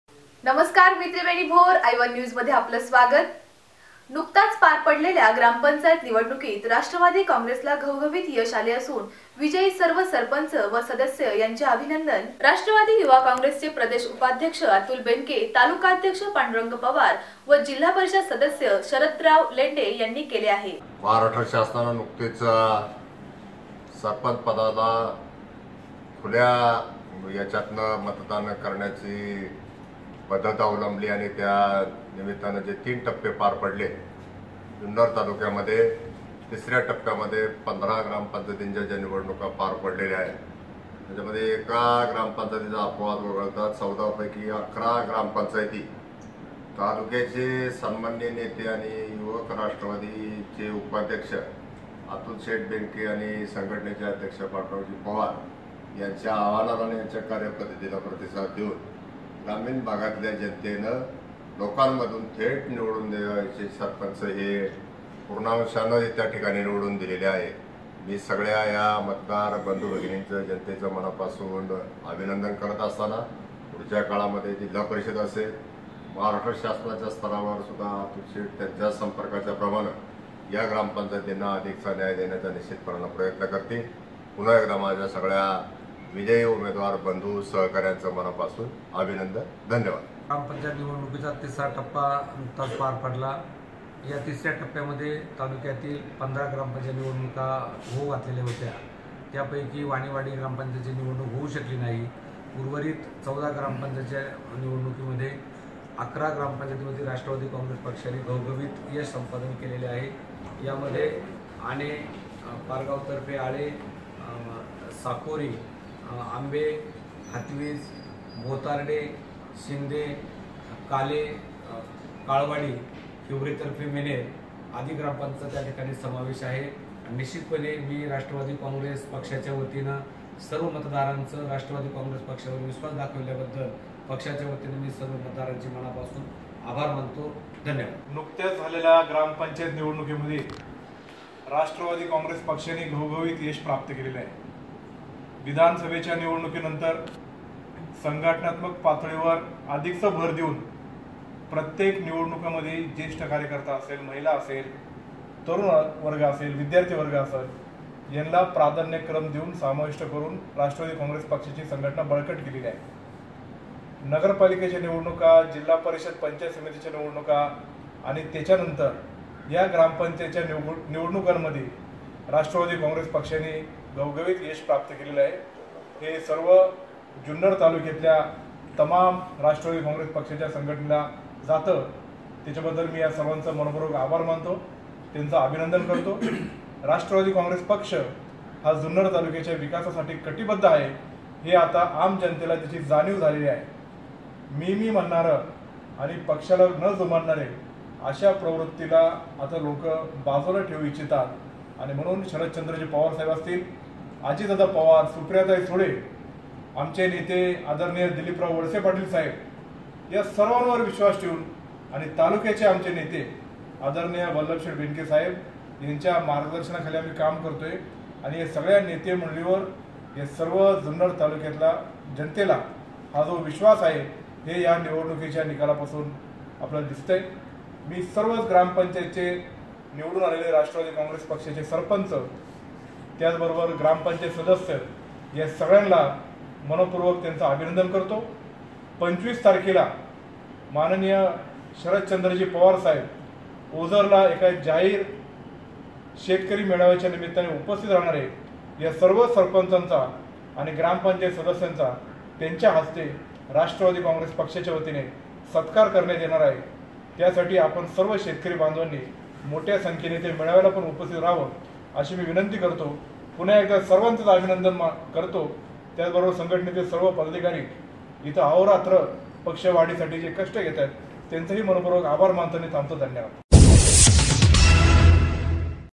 नुकत्याच पार पडलेल्या ग्रामपंचायत निवडणुकीत राष्ट्रवादी कॉंग्रेसला घावघणीत यश आले
नुकत्याच पार पडलेल्या ग्रामपंचायत निवडणुकीत राष्ट्रवादी कॉंग्रेसला घावघणीत यश आले असून विजयी सर्व सरपंच व सदस्य यांचे अभिनंदन..
राष्ट्रवादी युवा काँग्रेसचे प्रदेश उपाध्यक्ष अतुल बेनके,तालुकाअध्यक्ष पांडुरंग पवार  जिल्हा परिषद सदस्य शरदराव लेंडे यांनी केले आहे.